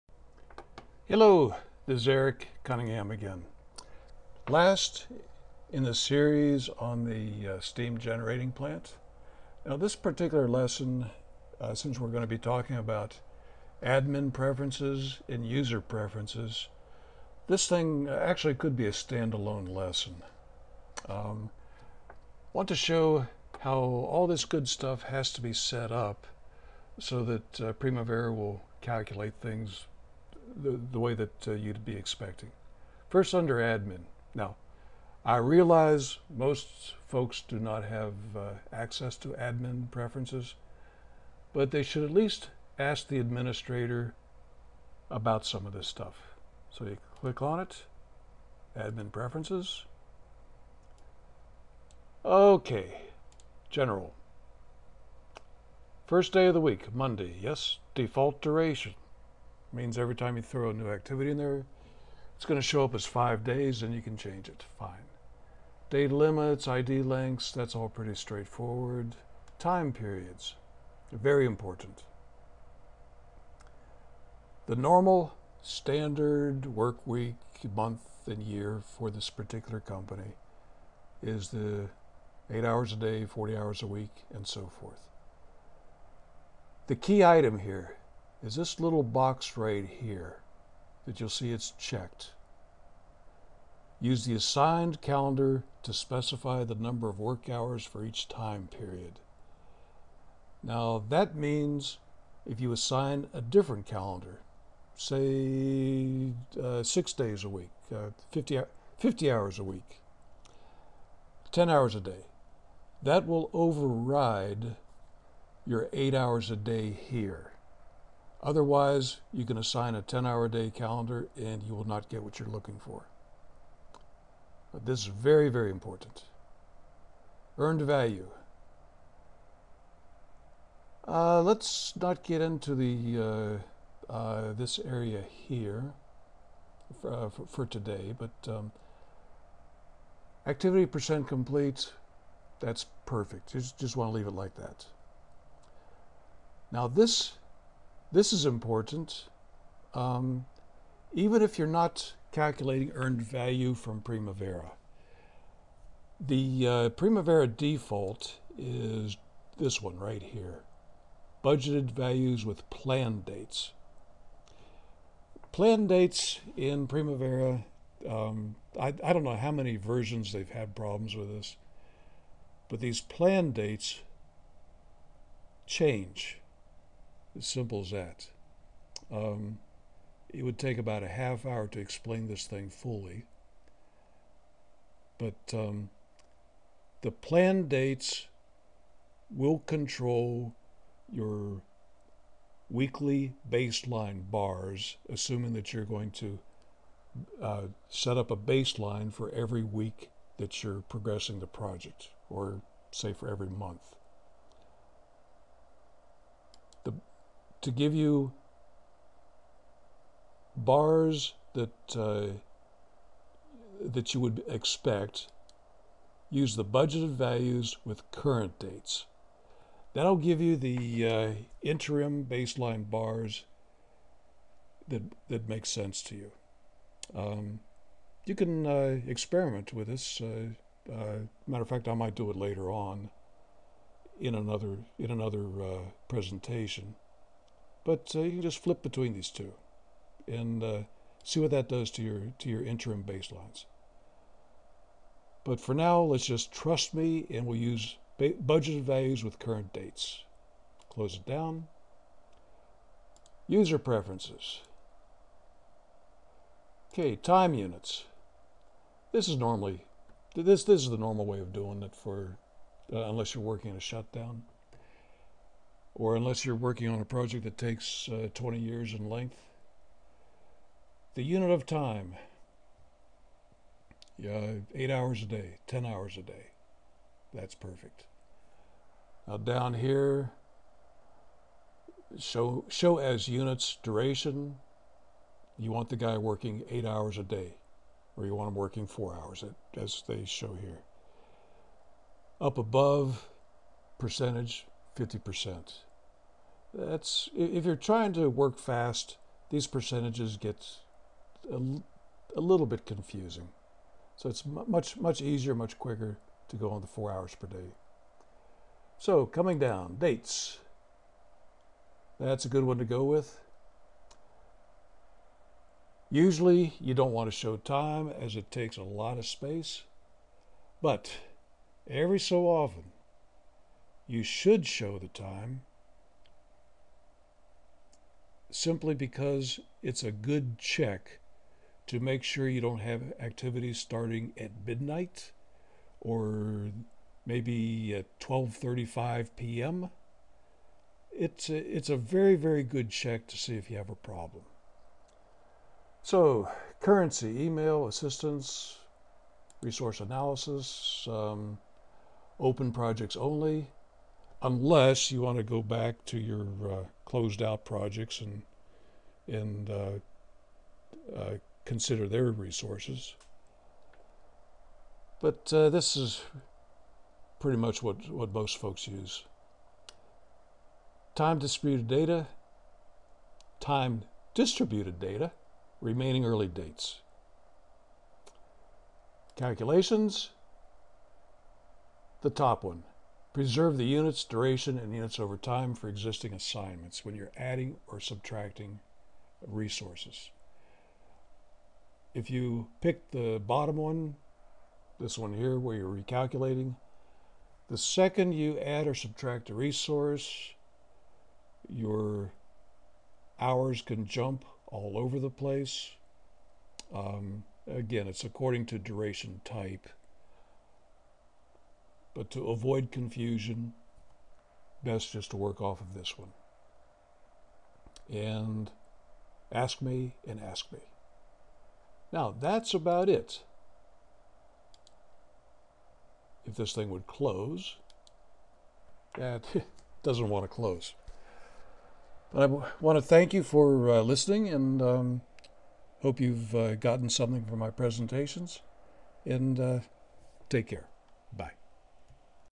Primavera P6 Steam Generating Plant Part 3 of 3
Proper setup of Admin Preferences, including Planned vs Current Dates to control interim baseline bars, setup of User Preferences.